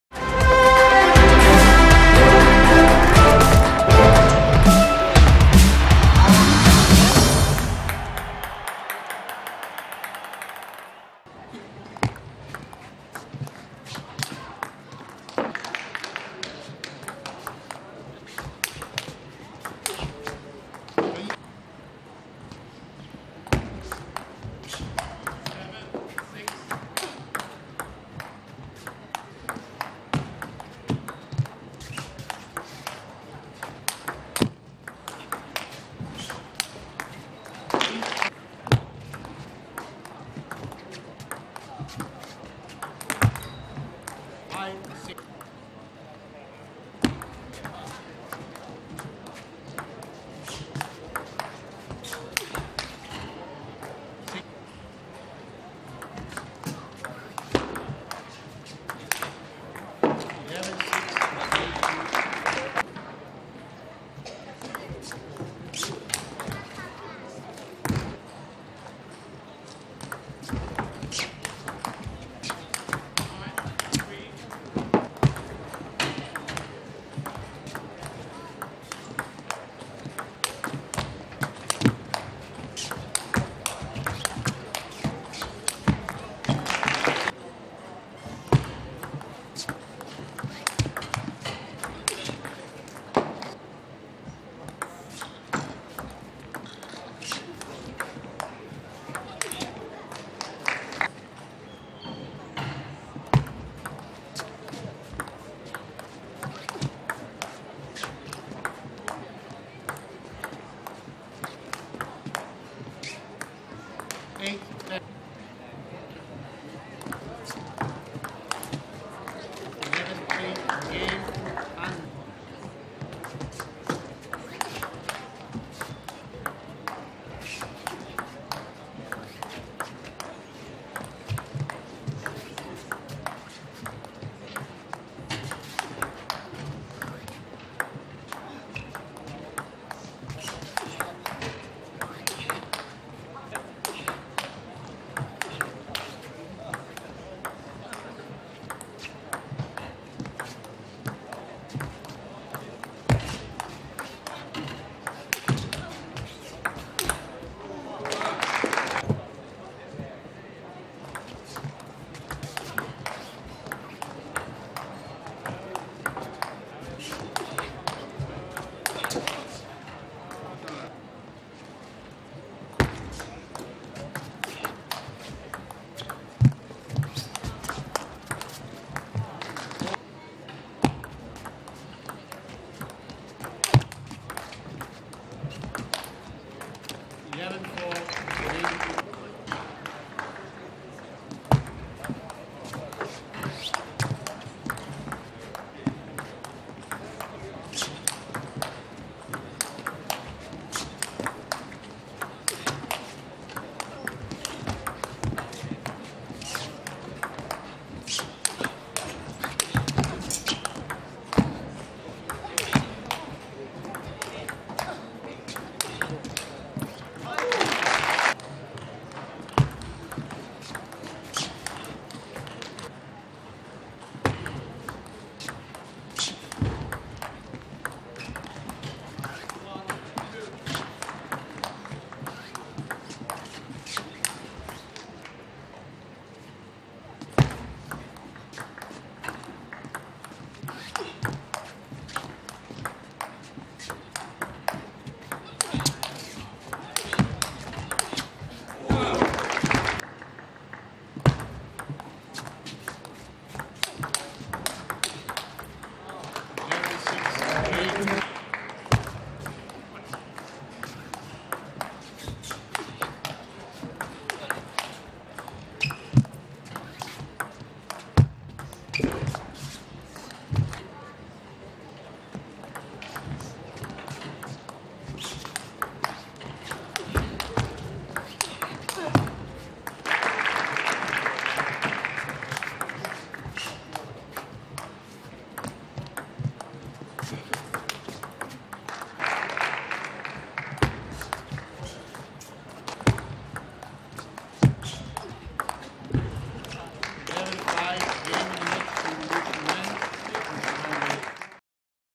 Swedish Open 2015 Highlights: LIU Shiwen vs HAN Ying (1/4)
Review all the highlights from the LIU Shiwen vs HAN Ying (1/4) from the Swedish Open 2015